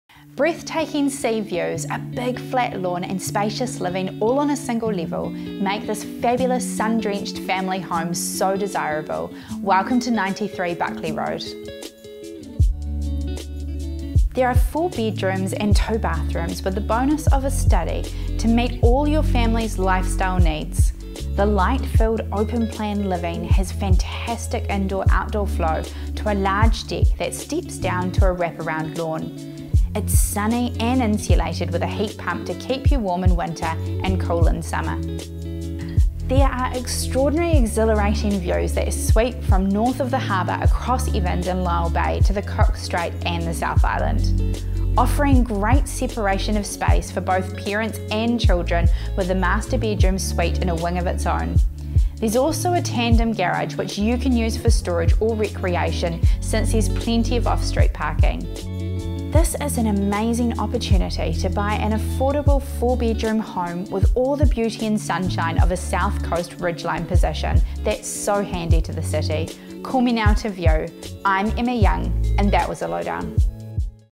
93 Buckley Road, Southgate - Wellington (Emma Young & Craig Lowe)
Breathtaking sea views, a big flat lawn, four bedrooms and spacious living all on a single level, make this a fabulous sun drenched family home that overlooks the Inner Harbour, the Harbour Entrance, and the South Coast so desirable! Its safely set back from the road, on the bus route, close to Island Bay village with its shops and cinema, and a short drive into the city. Features include:

- Four bedrooms, two bathrooms and the bonus of a study to meet all your family's lifestyle needs
- Light-filled open-plan living with fantastic indoor-outdoor flow to a large deck that steps down to flat wraparound lawn on which to spend hours of fun with your kids
- Sunny and insulated with a heat pump to keep you warm in winter and cool in summer
- Extraordinarily exhilarating views that sweep from north of the harbour, across Evans and Lyall Bays, to Cook Strait and the South Island
- Great separation of space for parents and children, with the master bedroom suite in a wing of its own
- Tandem garage which you can use for storage or recreation since there's also plenty of secure off-street parking
- Buyer Enquiry Over $1,395,000

This is an amazing opportunity to buy an affordable four-bedroom home with all the beauty and sunshine of a south coast ridgeline position that is super handy to the city. Don't miss Sunday's viewing.

Tender closes at 12pm, Thursday 25th November 2021 at 8 Kent Terrace, Welling-ton (will not be sold prior)

During the Alert Levels Lowe & Co Realty will be practising safe workplace guidelines as issued by REINZ.